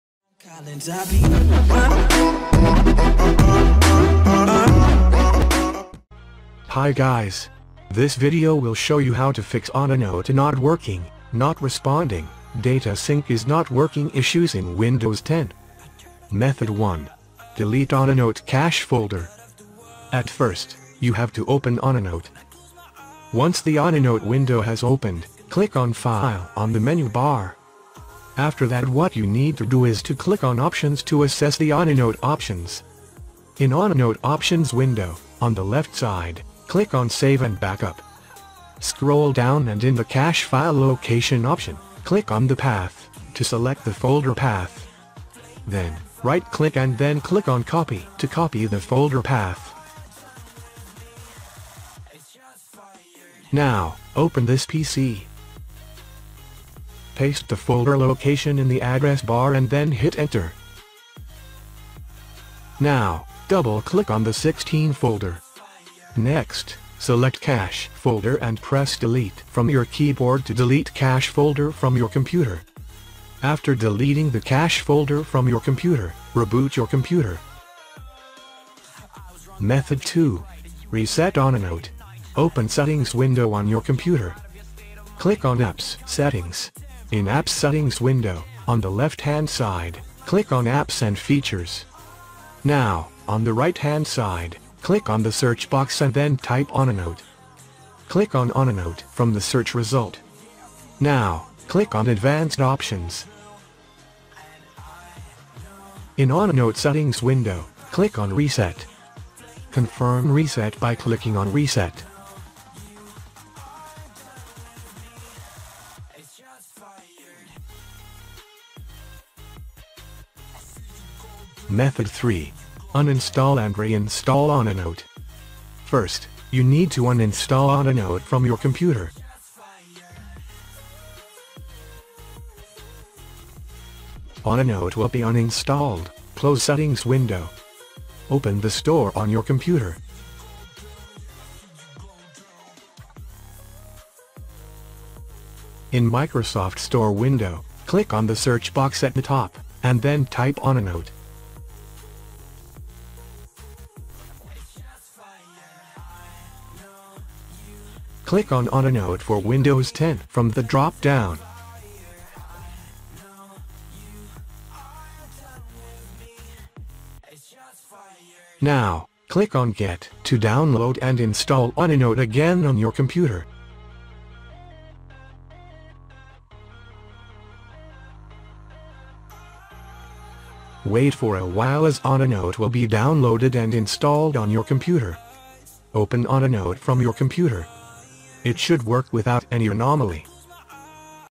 OneNote not responding, OneNote not working, data sync is not working in Windows 10 Fix
Hi guys! This video will show you how to fix OneNote not working, not responding, data sync is not working issues in Windows 10.
Method 1. Delete OneNote cache folder.
Method 2. Reset OneNote.

Method 3. Uninstall and Re-install OneNote